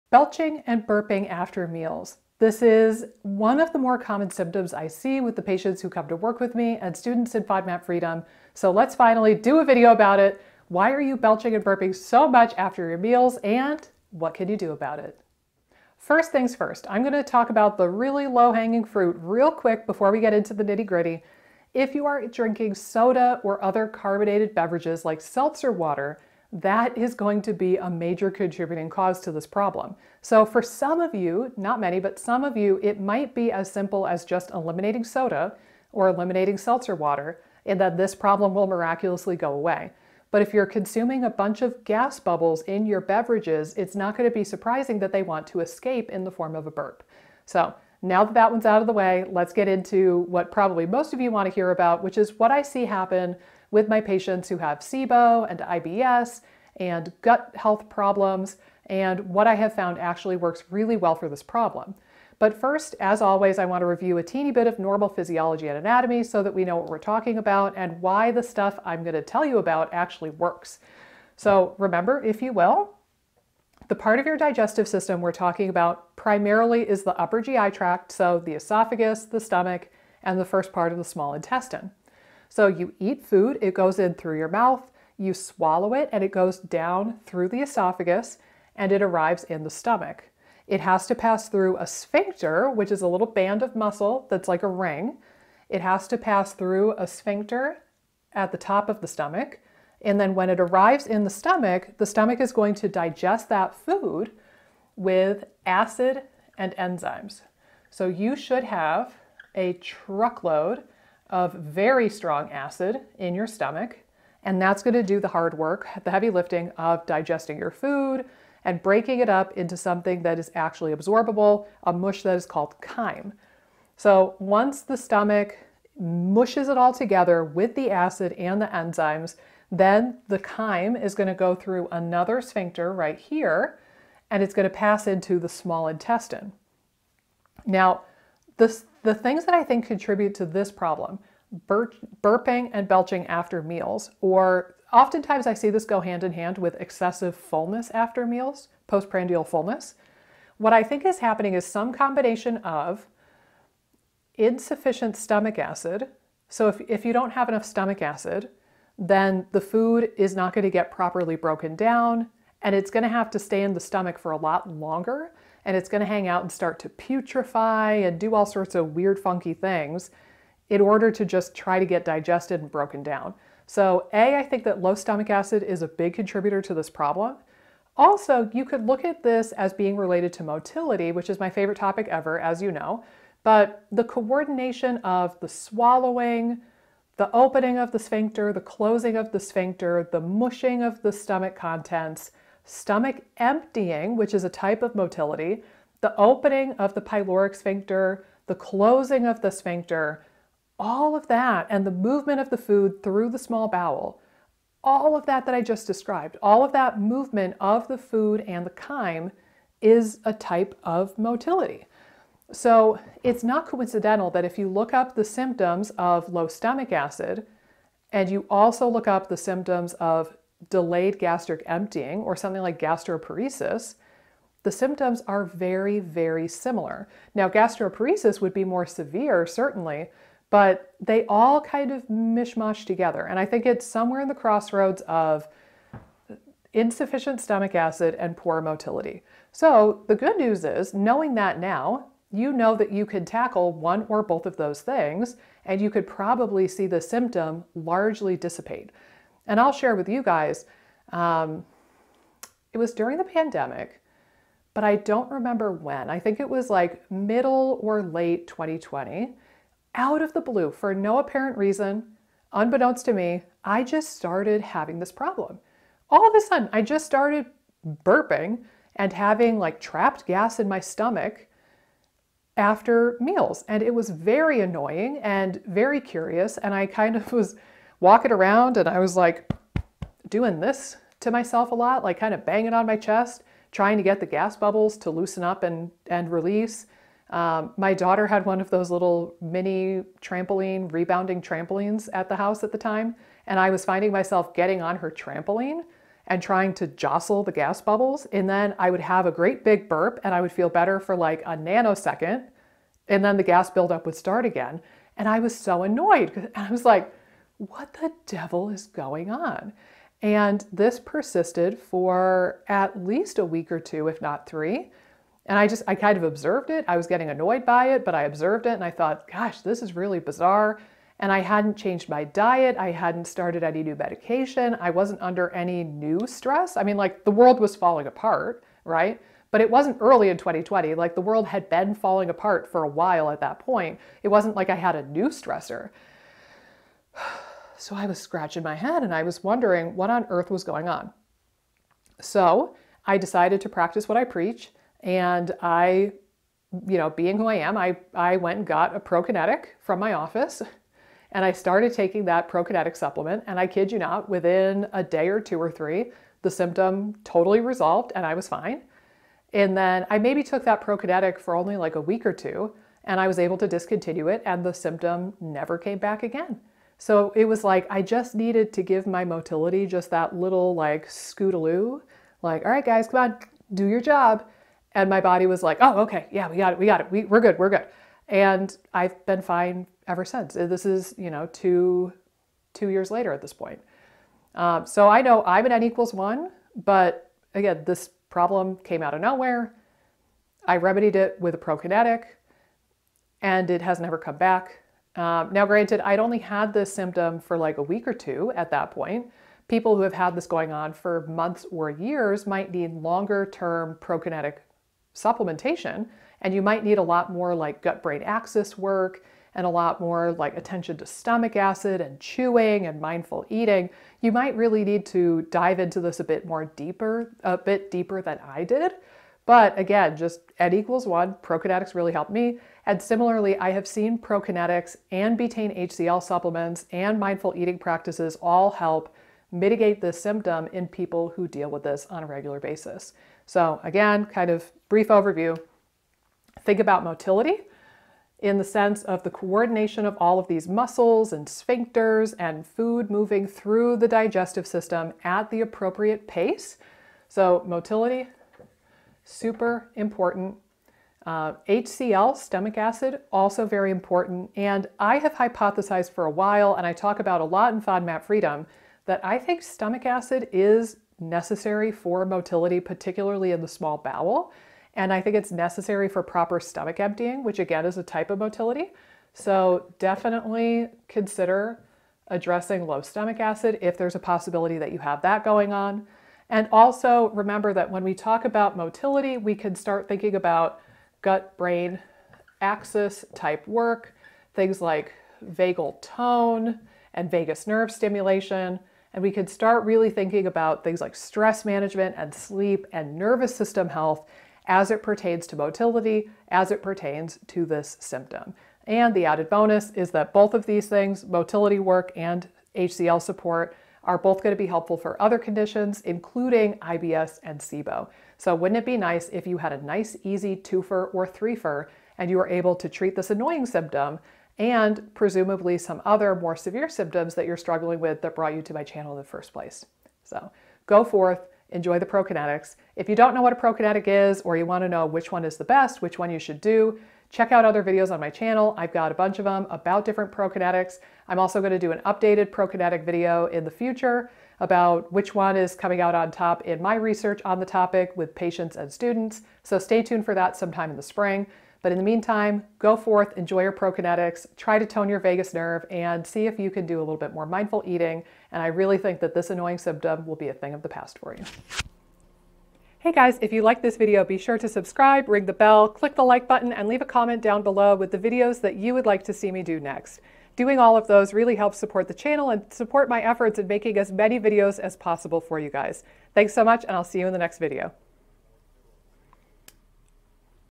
Burping and Belching After Meals
Have you ever experienced Burping and belching too much after meals? Is burping and belching connected with IBS and SIBO? Learn more about the reasons for too much burping and belching and ways you can lessen it in this video!

Dr. Nicole DiNezza, DC, NTP
Holly Springs, NC